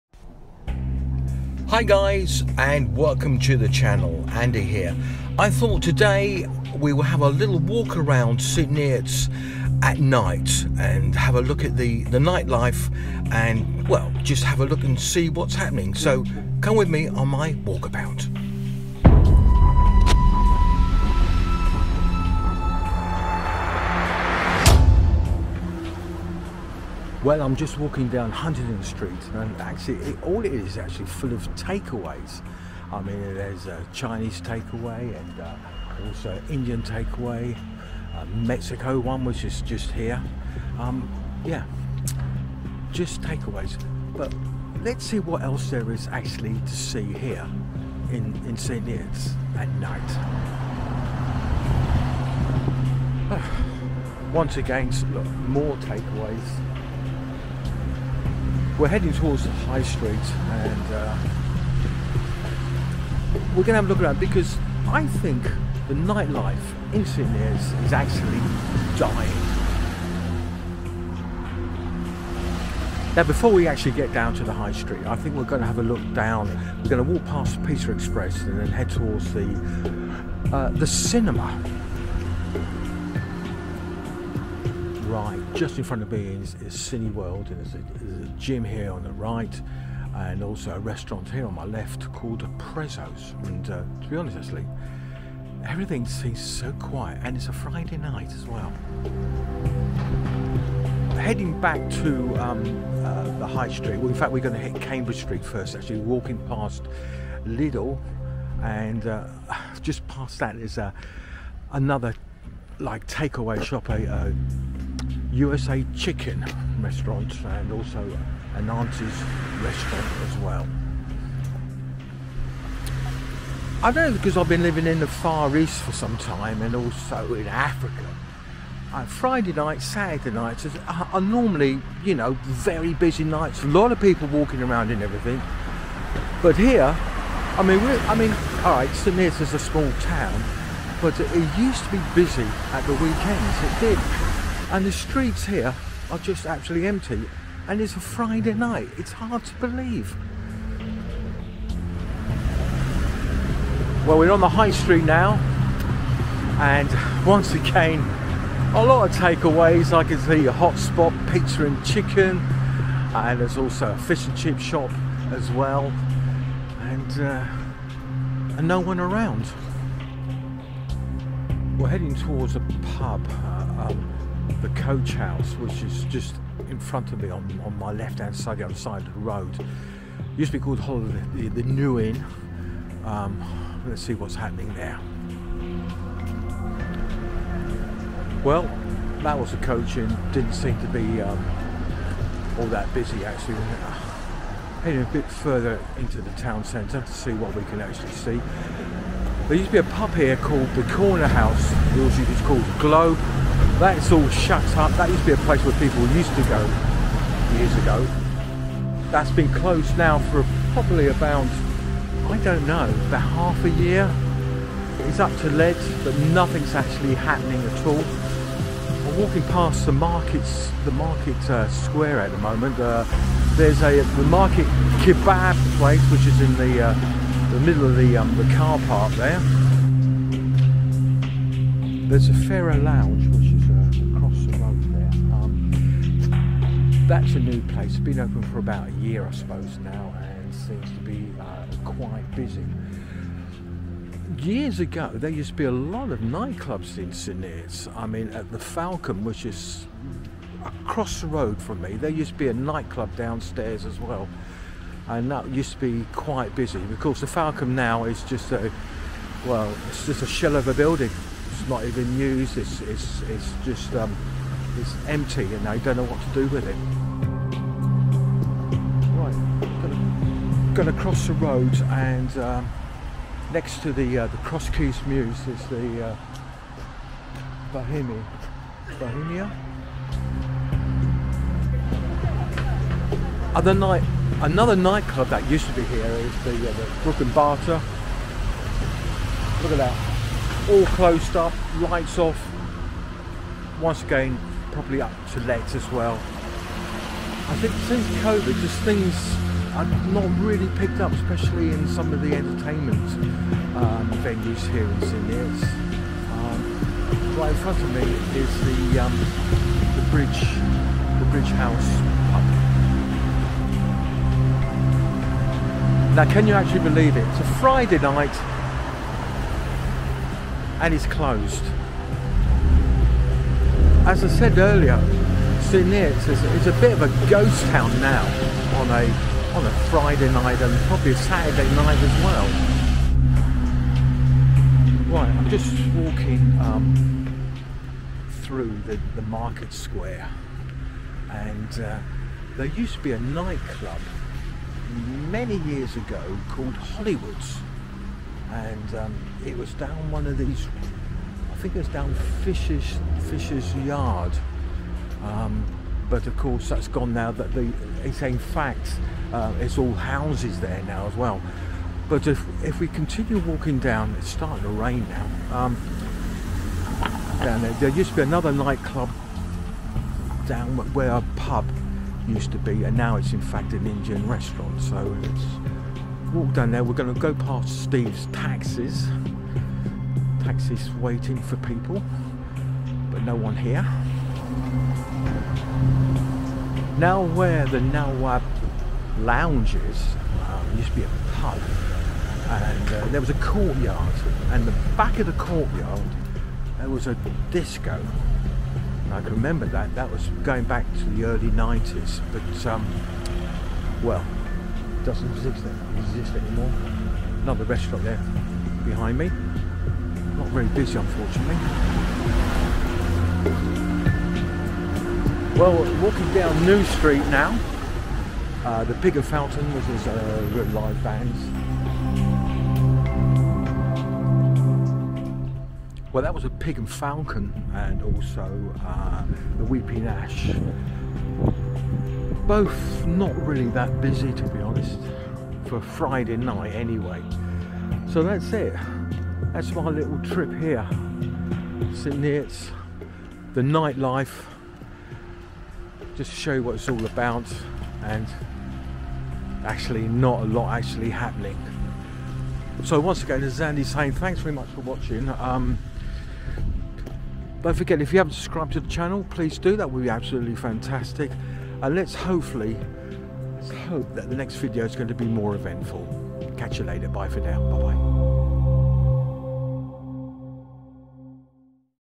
St Neots nightlife 2023
St Neots nightlife 2023, you know what, I should have stayed at home. Come with me on my walk about around St Neots on a Friday night. I know it had been raining, but come on it’s a Friday night. Half the venues were closed up, and the other half  were empty. Now, I remember when a Friday night in St Neots was for fighting, well you know what I mean. There was several small nightclubs and all were busy. Times gave certainty changed. Anyway, come with me and let us both be disappointed lol. Cheers Andy.